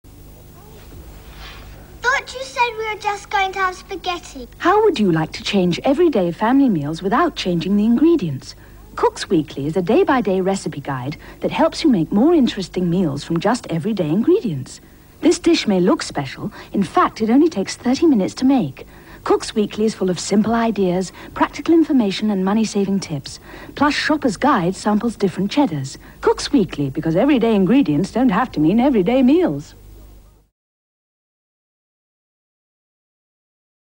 Cook's Weekly | Advert | May 1985
NEW. An advert for Cook's Weekly magazine for all housewives in the 197... I mean.. 1985. Yes believe it or not, this is an advert for an era where cod fritters and cheddar cheese is all the rage! No idea what a 'cauliflower mexicana' is but.. erm.. I'd leave it a few minutes before going in there. !